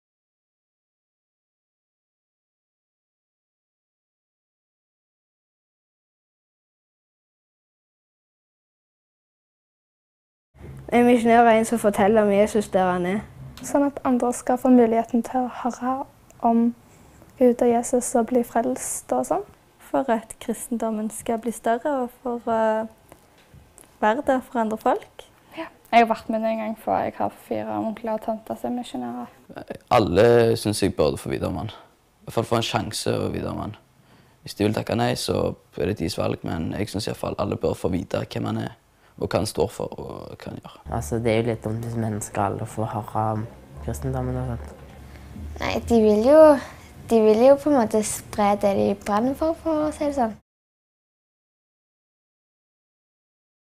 Tanker om tro 9 -Misjon
Hvem er Jesus? Hva er nattverd? Hvorfor har vi de ti bud?
Disse og flere spørmål stilte vi konfirmantene i Vardeneset menighet da de var på konfirmantweekend i begynnelsen av året. Resultatet ble ni korte snutter hvor konfirmantene forteller åpent om ulike temaer relevante for ungdommer.